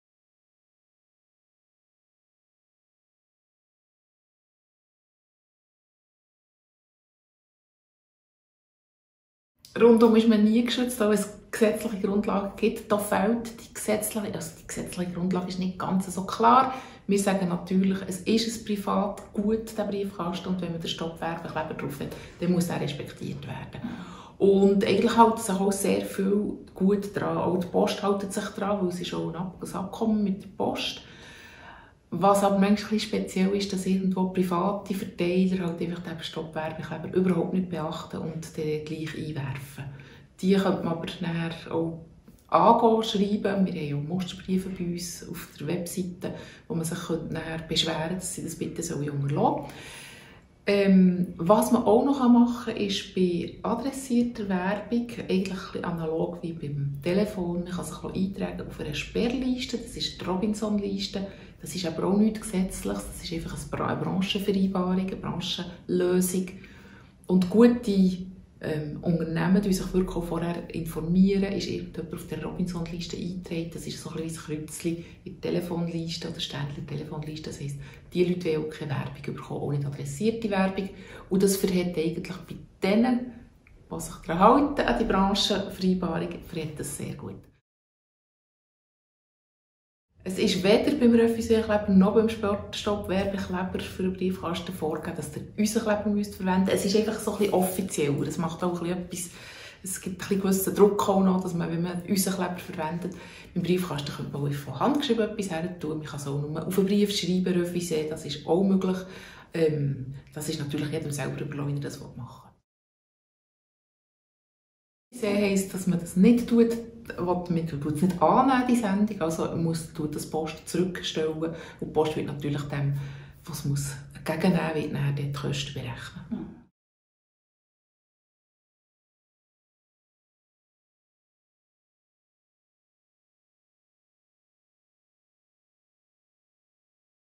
Entsorgung + Recycling Stadt Bern: Stopp-Werbung-Kleber
Wie effizient ist der Stopp-Werbung-Kleber? Und was kann man gegen adressierte Werbung tun? Interview mit Sara Stalder, Geschäftsleiterin der Stiftung für Konsumentenschutz.